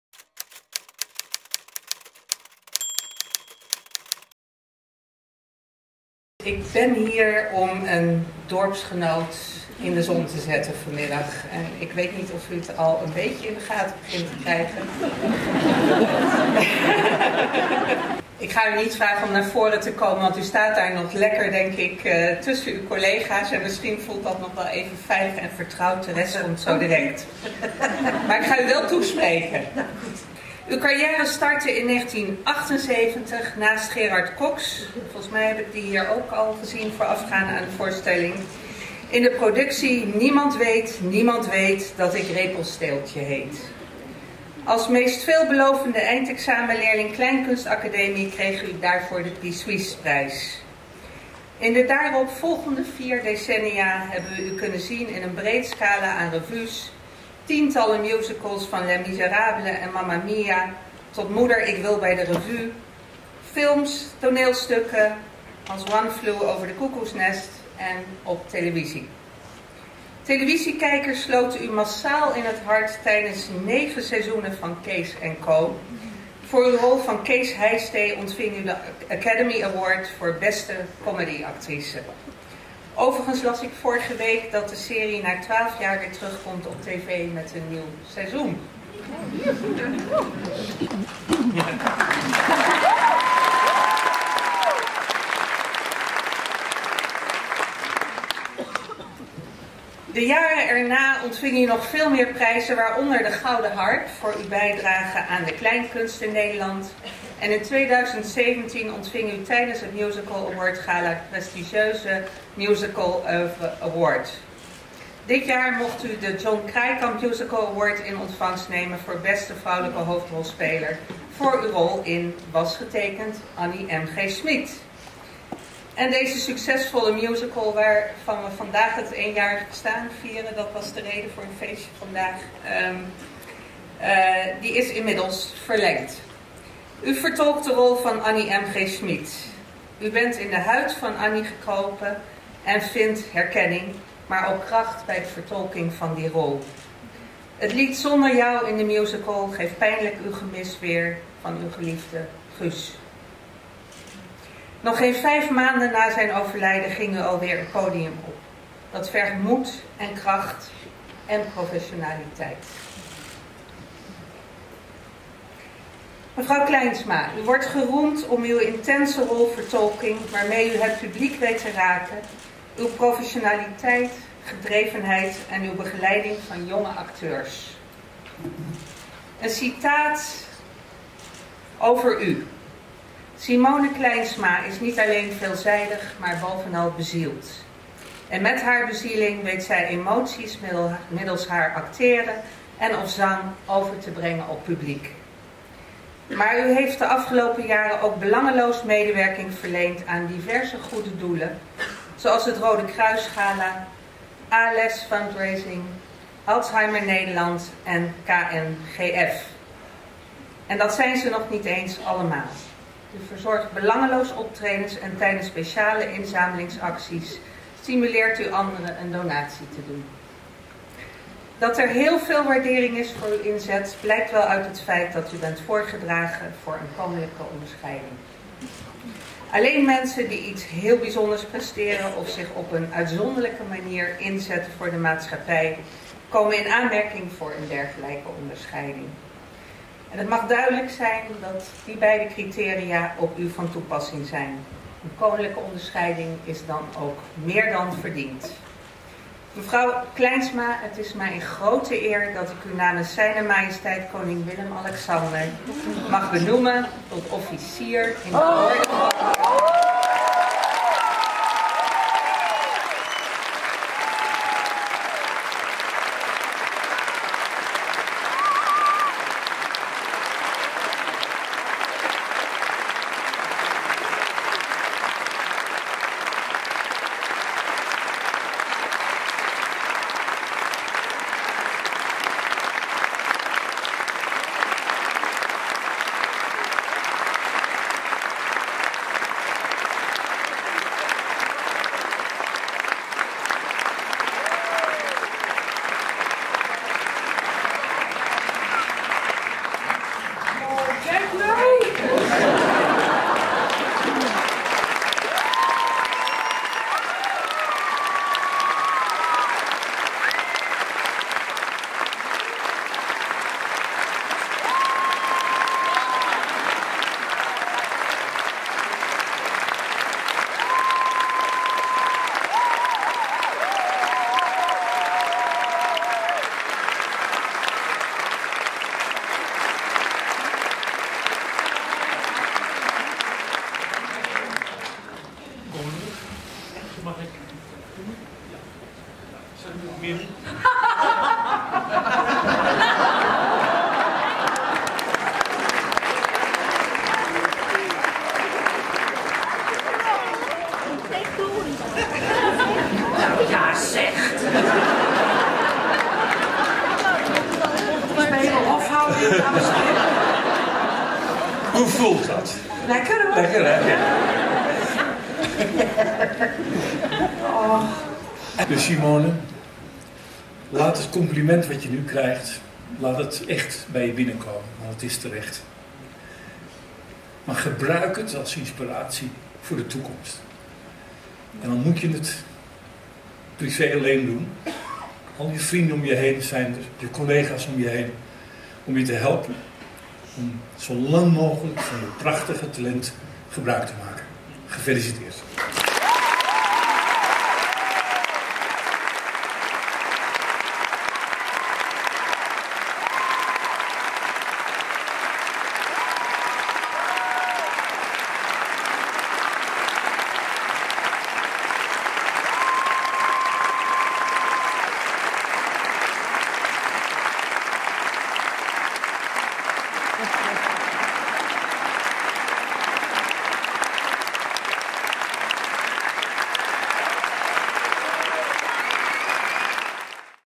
Een Koninklijke onderscheiding voor Simone Kleinsma
Op zondag 30 september vierde 'Was Getekend, Annie M.G. Schmidt' niet alleen dat de voorstelling al een jaar met veel succes gespeeld werd, er was ook nog een bijzonder moment voor hoofdrolspeelster Simone Kleinsma.